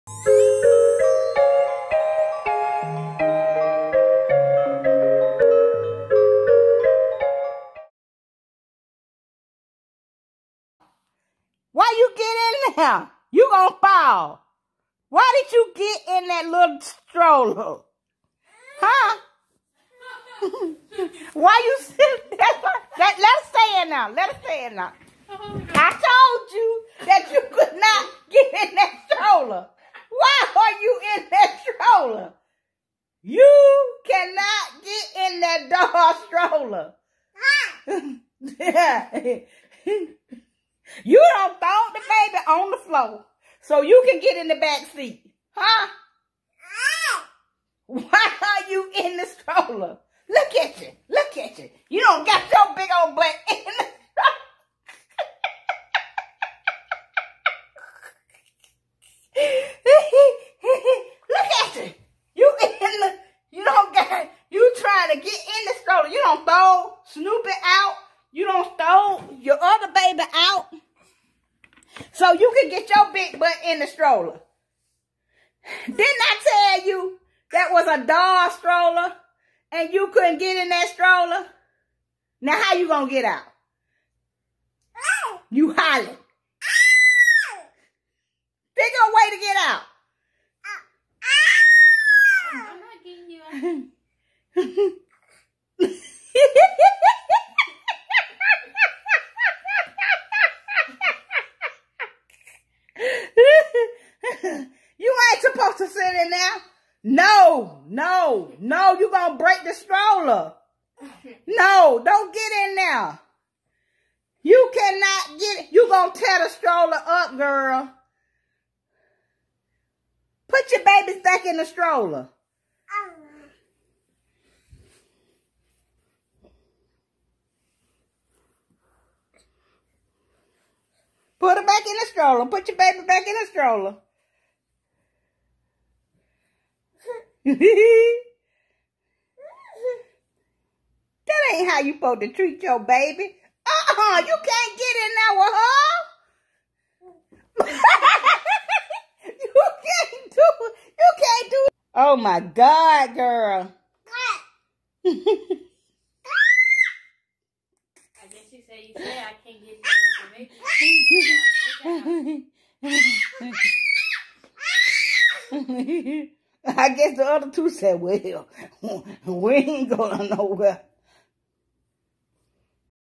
Apple and The Toy Stroller: Epi 3| Apple’s Adventures
Here's a another Adventure with Apple!! She love finding new and old toys to play with, in this episode Granny Giant brings by a toy stroller that Big Apple (Apple's MOM) played with as a child and Apple decides she want to do more than just play with the stroller!!!